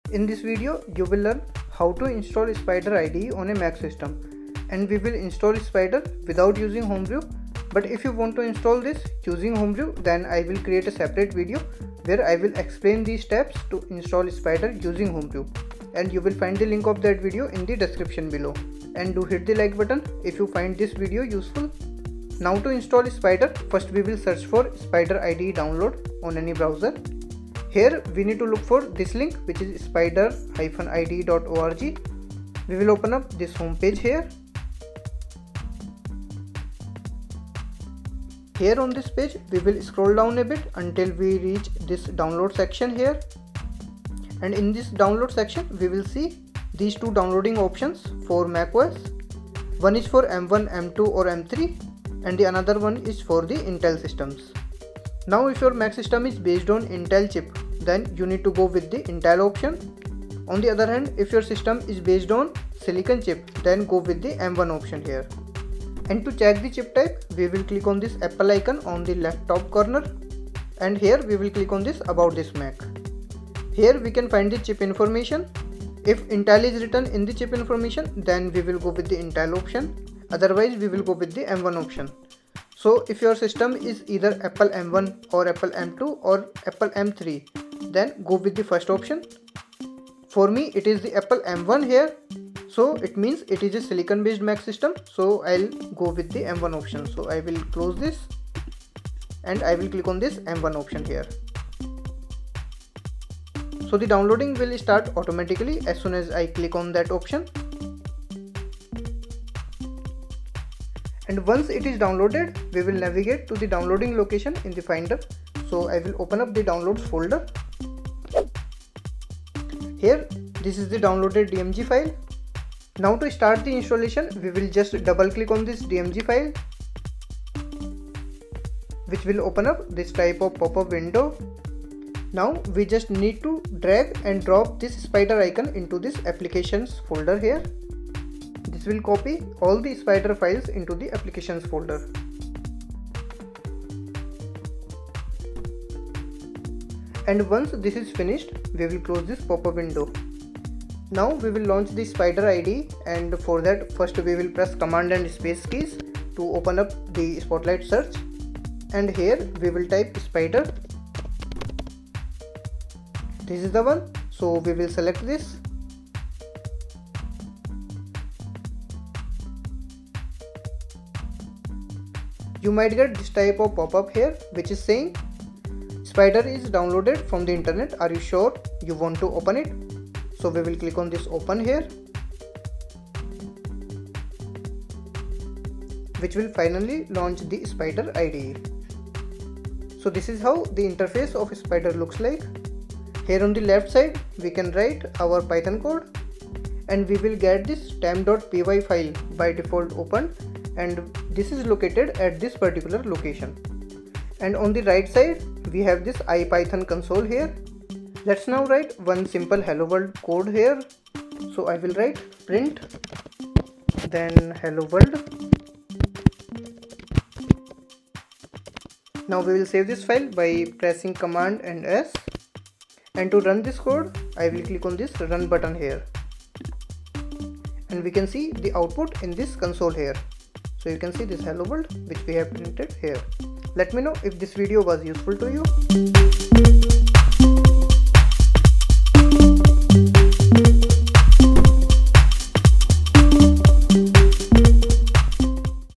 How to Install Spyder Python in Mac | How to Download & Install Spyder on Mac
I have explained, how to easily download & install Spyder Python IDE in Mac OS (Macbook, Mac M1 / M2 / M3, Apple M1, Pro).

After installing Spyder on Mac system, I have also given example of simple "Hello World" program in that Spyder IDE.

Welcome to our comprehensive guide on how to install Spyder Python IDE on your Mac! In this tutorial, we will walk you through each step to download and set up Spyder on macOS, making it easy for you to start coding in Python.

🔍 In this video, you will learn:
Where to download Spyder Python IDE for Mac.
Detailed steps to install Spyder on macOS.
How to configure Spyder for your Python development needs.
Tips to troubleshoot common installation issues.

Spyder is an open-source Integrated Development Environment (IDE) that is perfect for scientific programming and data analysis in Python. It comes with powerful features such as an interactive console, advanced editing, and debugging tools.